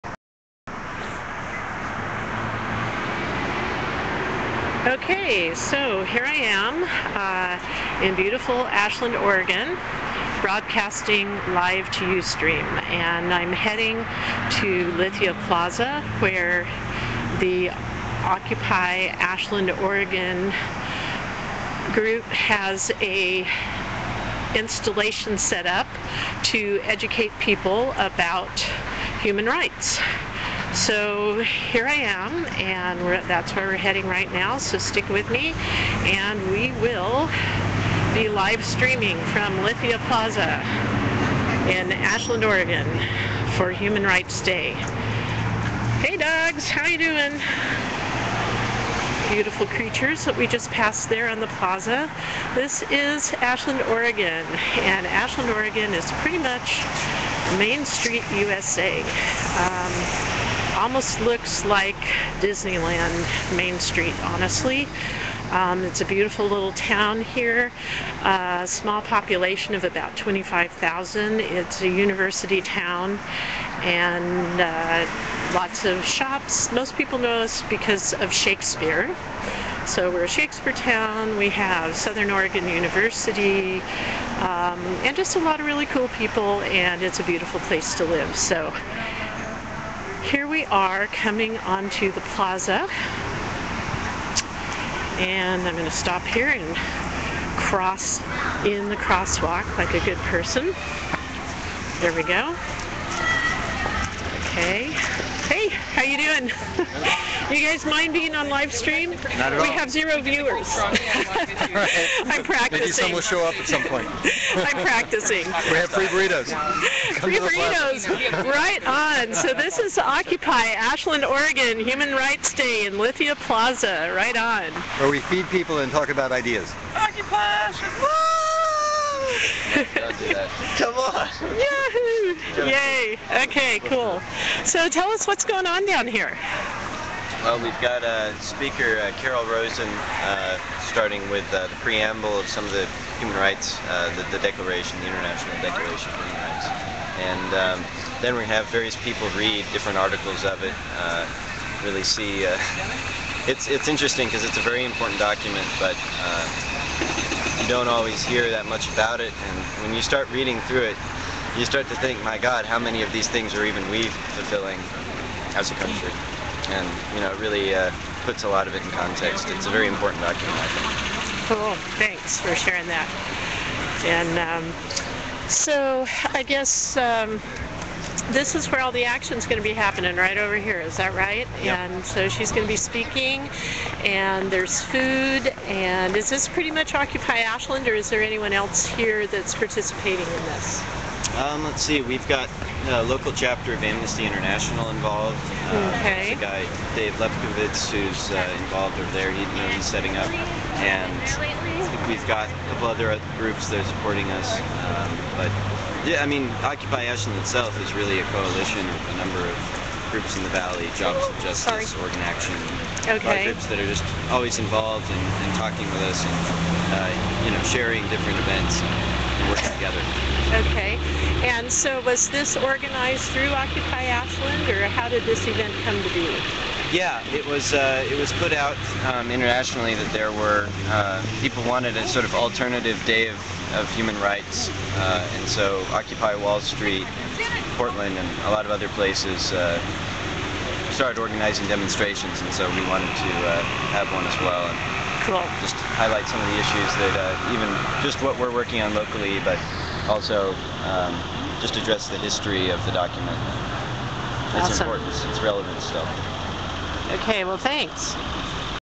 Human Rights Day (1)
Occupy Ashland celebrates Human Rights day.